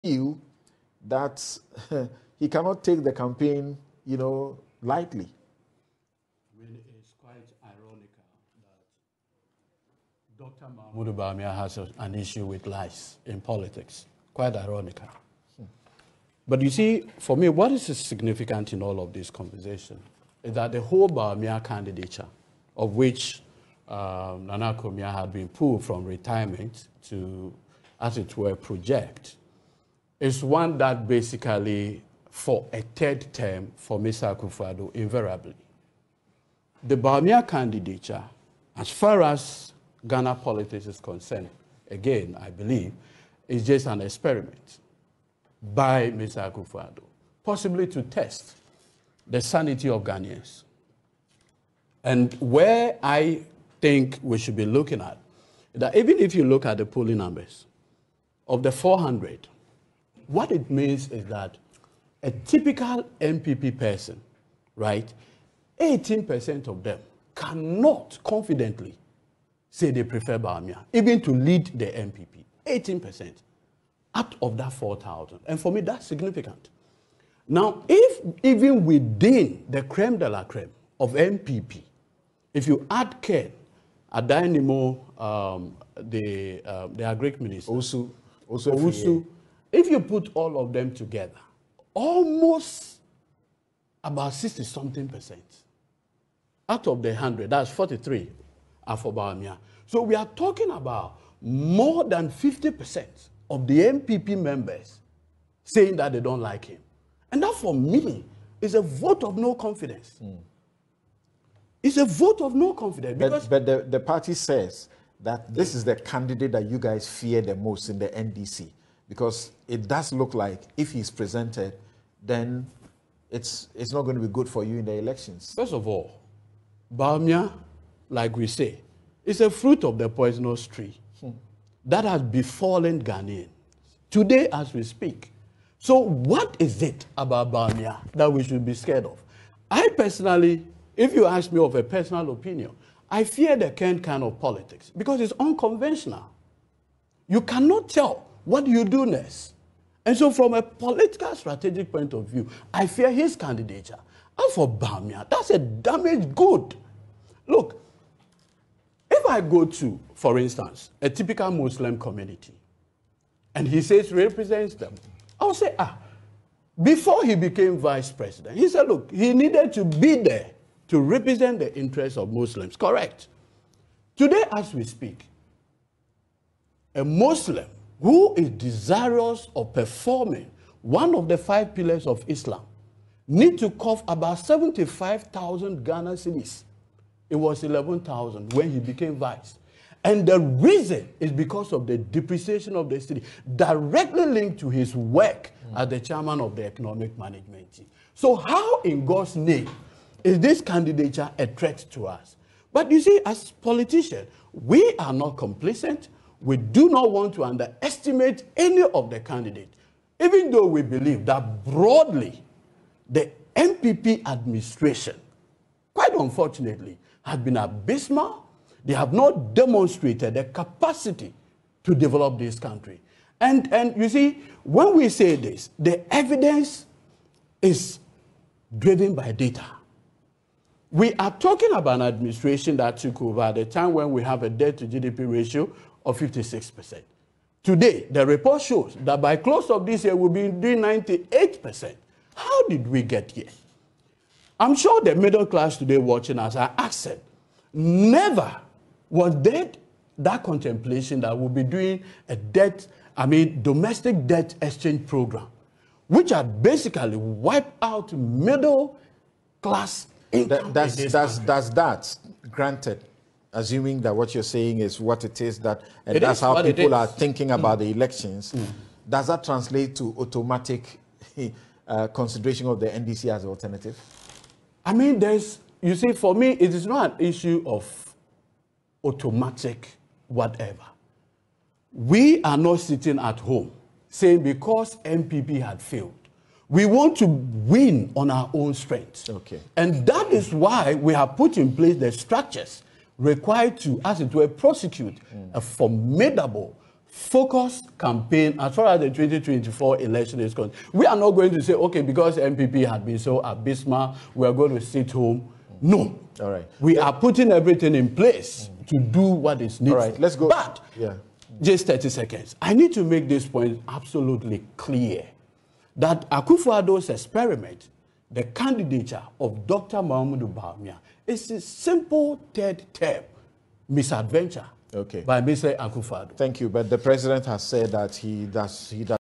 The whole Bawumia candidature is a third term for Nana Akufo Addo invariably - Godwin Edudzi Tameklo
"The whole Bawumia candidature is a third term for Nana Akufo Addo invariably"

- Godwin Edudzi Tameklo on Bawumia vs Ken.